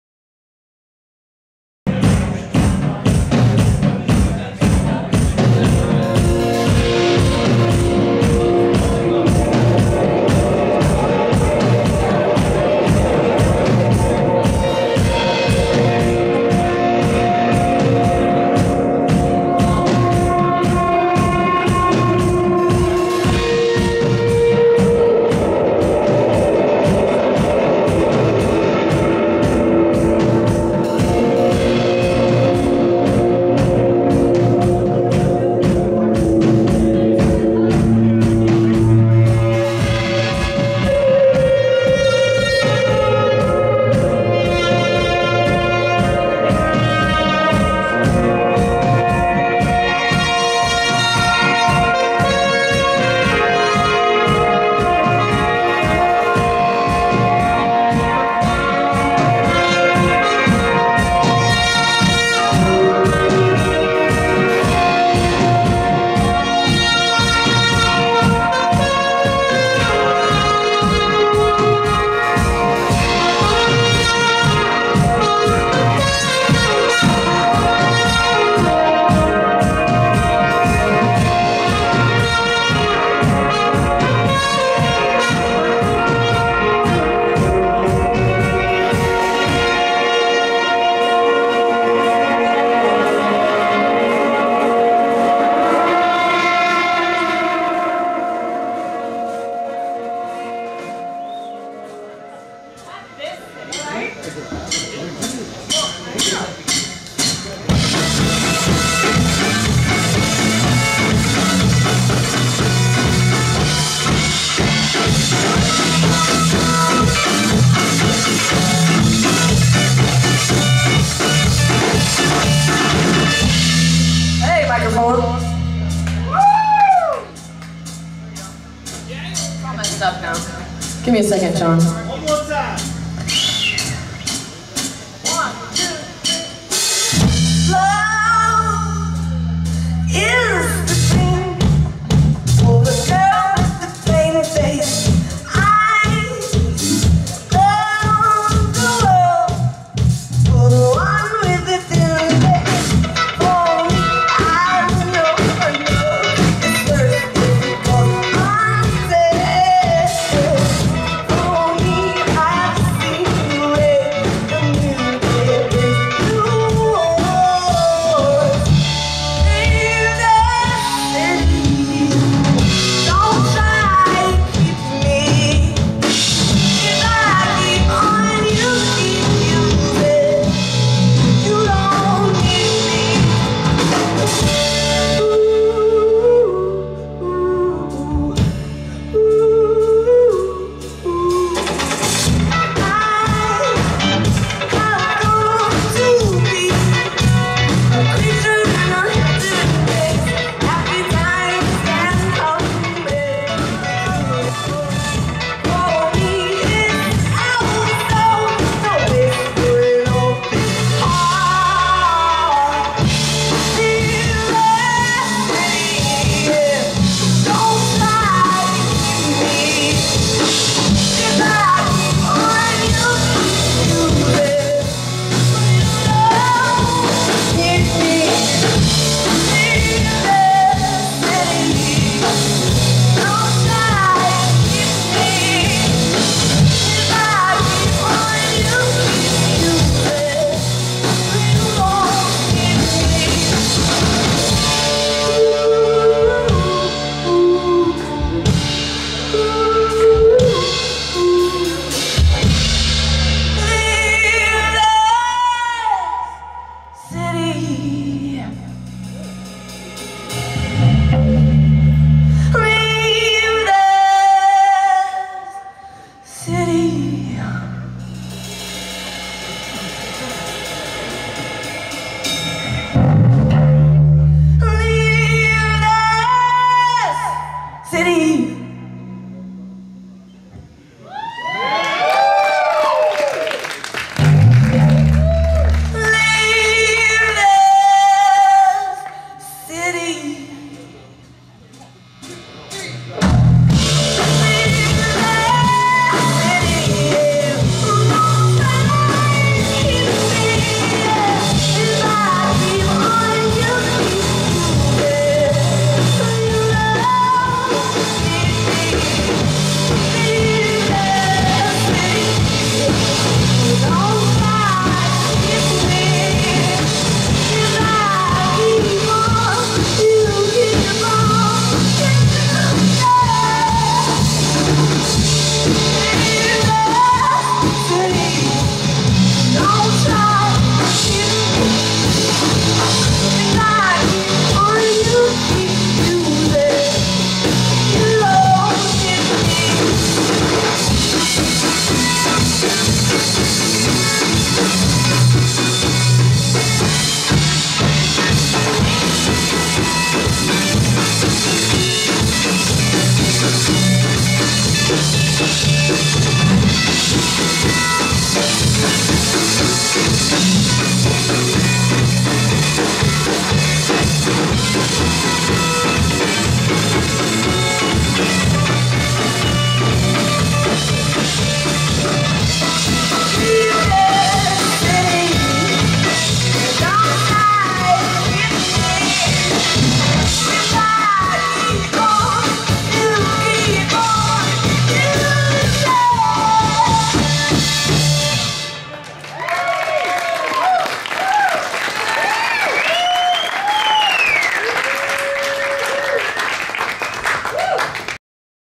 Kink Ador @ The Cutting Room
Live performance of the band Kink Ador
Location: The Cutting Room, New-York City
Shot & edited by Tanguy de Thuret
July 2014